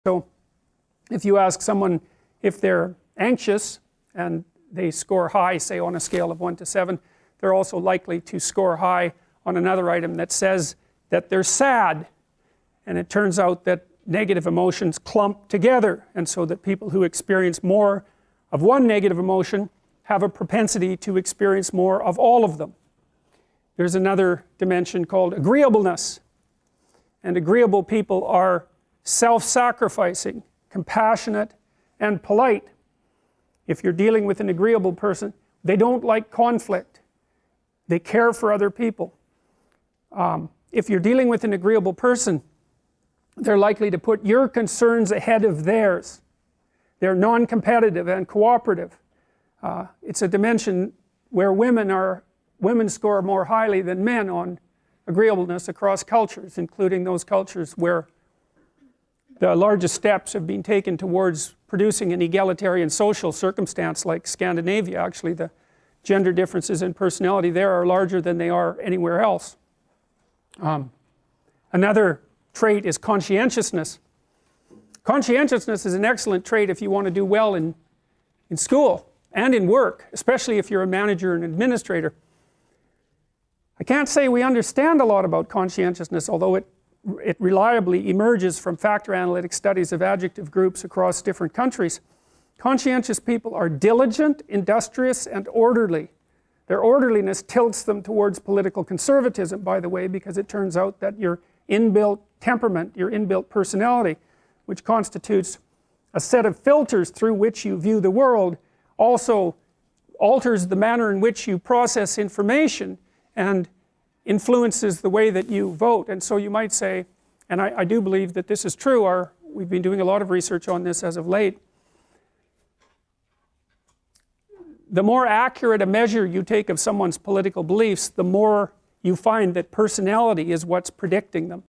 Personality Traits: Agreeableness, Conscientiousness, and Political Views
Dr. Jordan Peterson explores Big 5 personality traits agreeableness and conscientiousness, revealing how they influence social interactions and even political beliefs. Uncover insights into gender differences and the impact of personality on information processing.